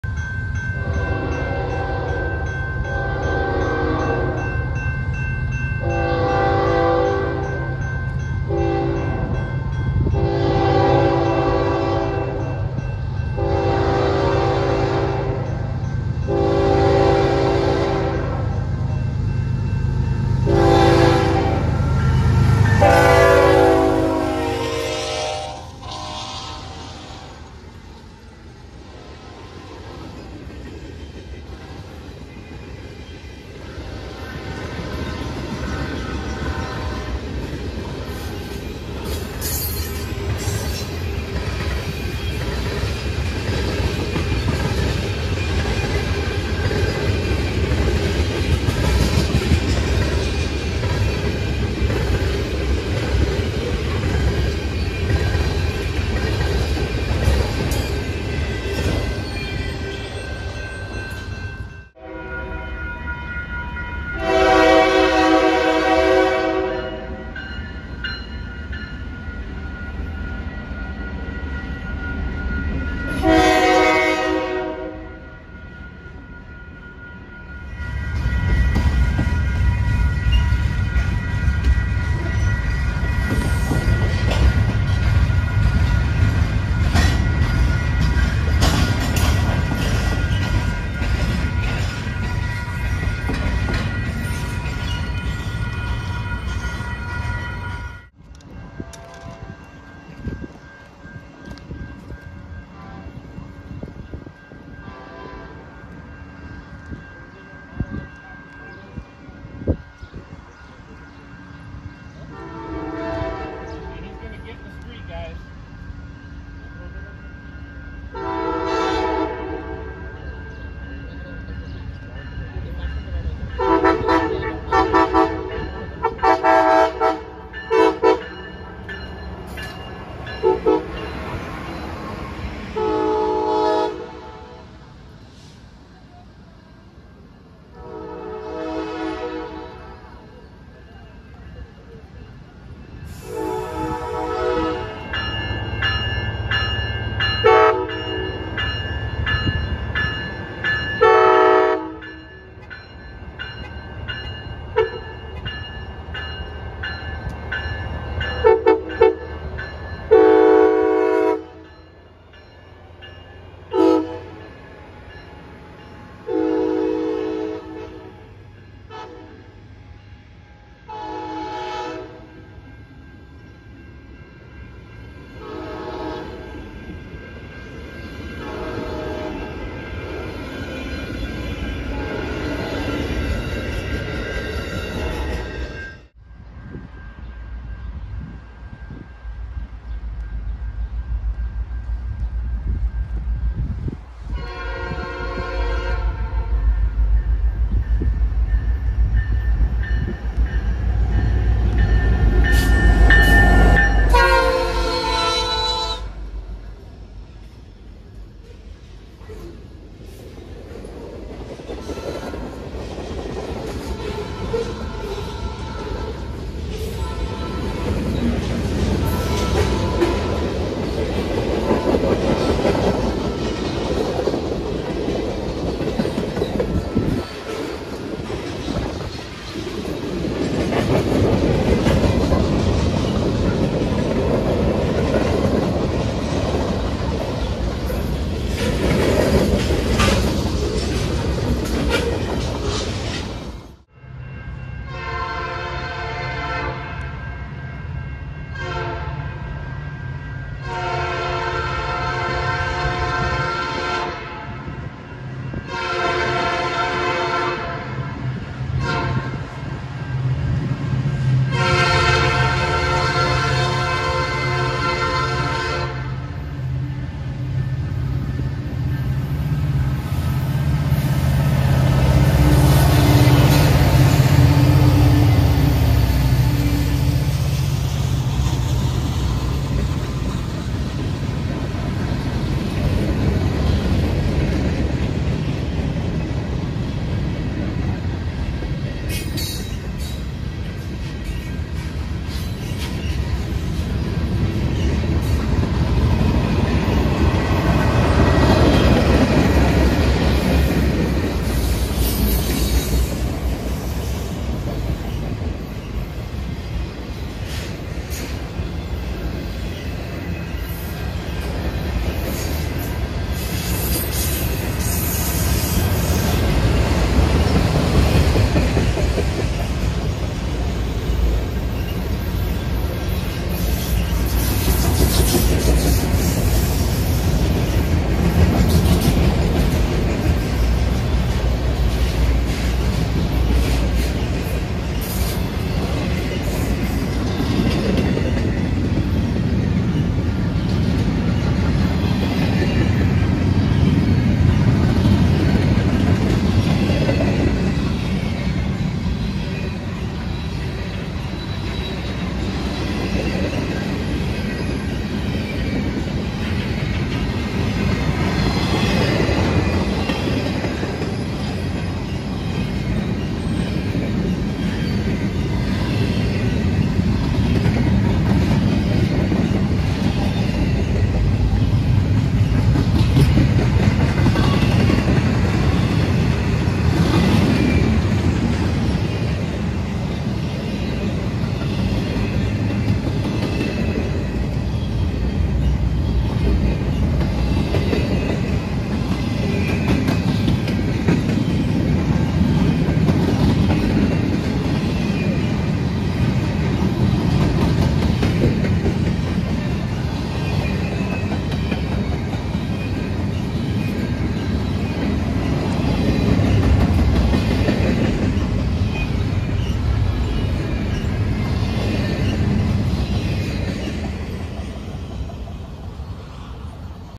Late afternoon railfanning on the Holly subdivision, Royal Oak, Mi
Filmed 7/8/24